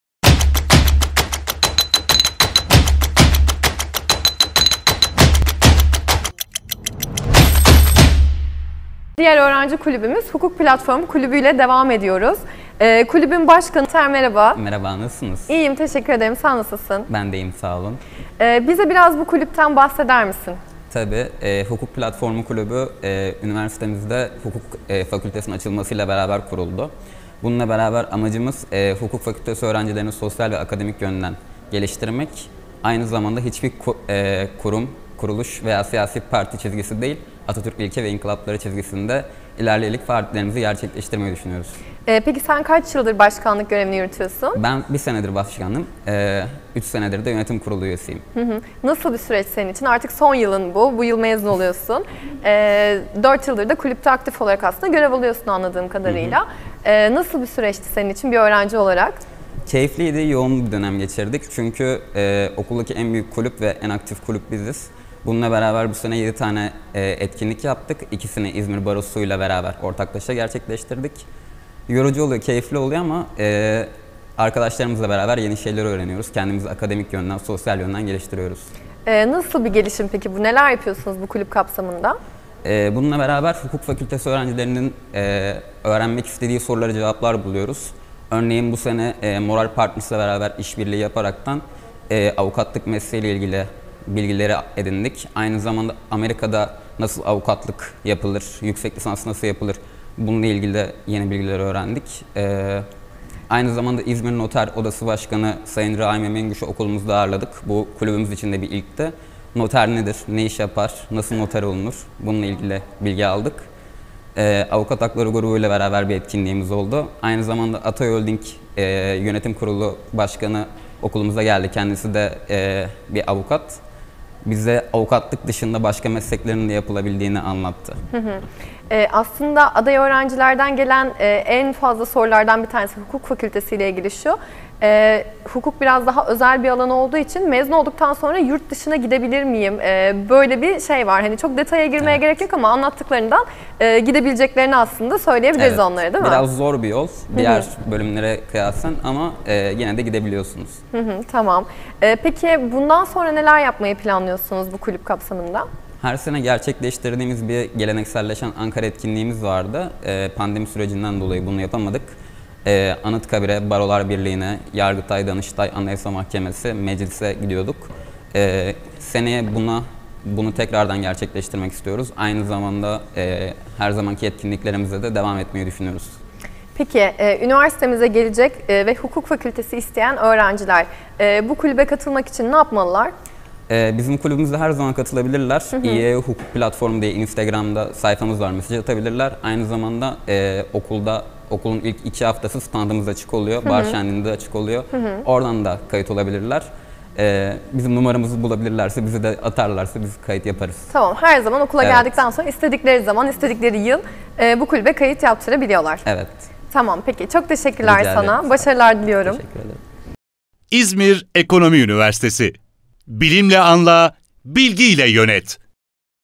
Hukuk Platformu Kulübü
İzmir Ekonomililerle 5 Dakika: Hukuk Platformu Kulübü
Kulüp Başkanı İlter Kutlu Denizhanoğlu sizler için anlatıyor.